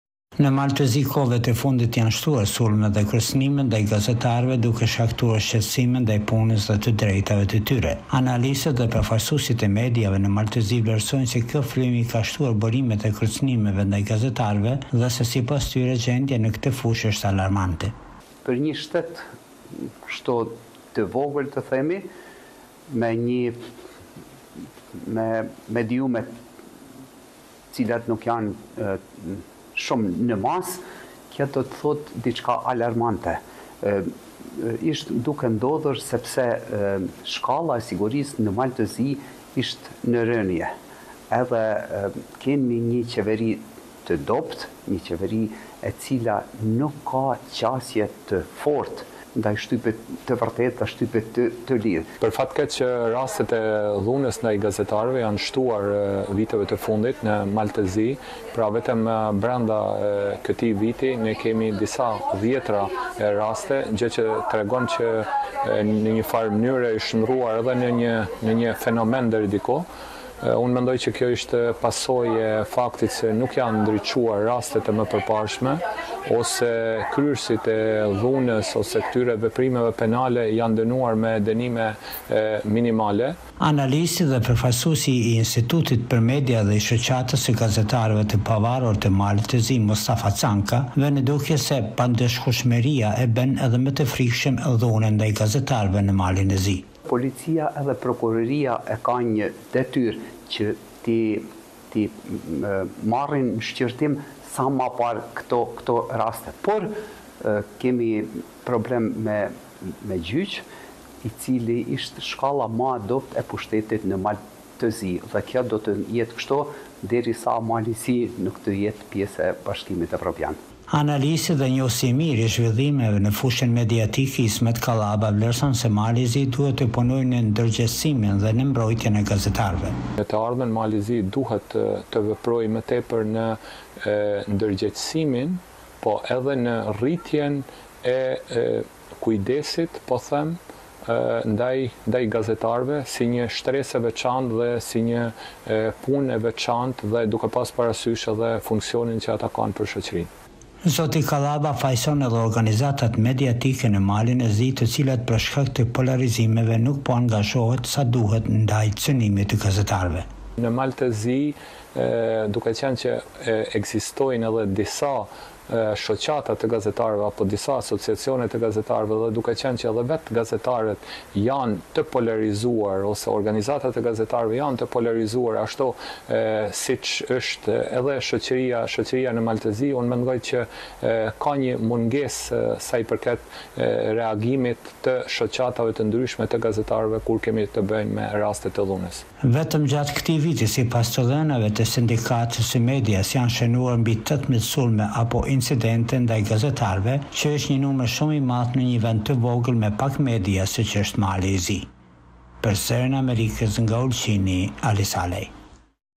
Analistët: Shtohet dhuna ndaj gazetarëve në Mali e Zi
Në Mal të Zi, kohëve të fundit, janë shtuar sulmet dhe kërcënimet ndaj gazetarëve duke shkaktuar shqetësime ndaj punës dhe të drejtave të tyre. Analistët dhe përfaqësuesit e mediave në Malin e Zi vlerësojnë se gjendja në këtë fushë është alarmante.